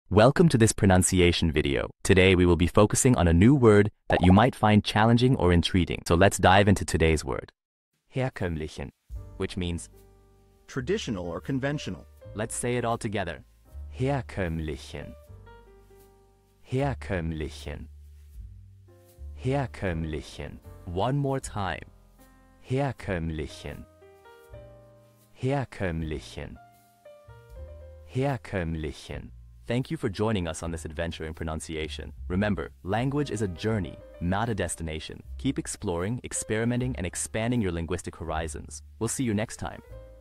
How to pronounce Herkömmlichen
Master the Pronunciation of 'Herkömmlichen which means conventional' - which means : Traditional or conventional. 📝 with @PronunciationGerman 🎓🗣️ - [Your Guide to German]

Learn to pronounce 'Herkömmlichen' perfectly in German with our easy and engaging tutorial by @PronunciationGerman! 📖📽️ In this comprehensive video, we provide you with native speaker examples 🗣️👂 and phonetic breakdowns 🎵 to ensure you get the pronunciation just right.

How to say Usual in German